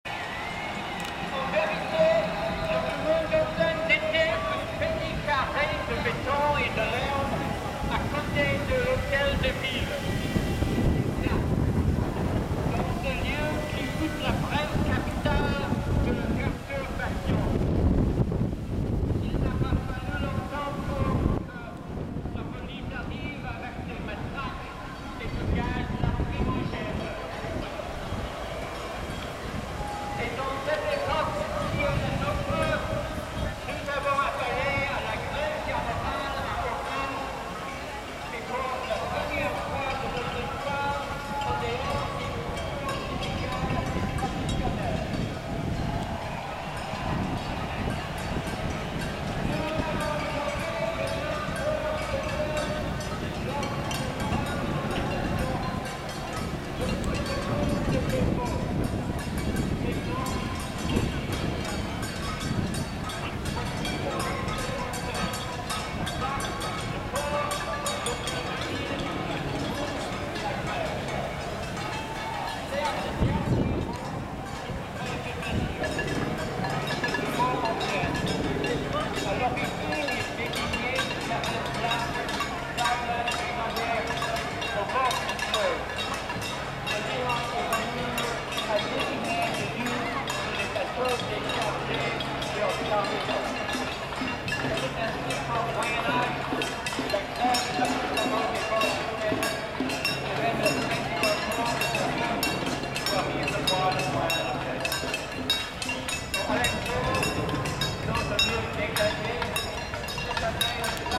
(11) Montréal June 22, 2012 Student Movement March of 75,000
Mouvement Étudiantes-s marche de 75,000 le 22 juin 2012 contre la hausse de frais de scolarité et contre la loi 78 d'état policier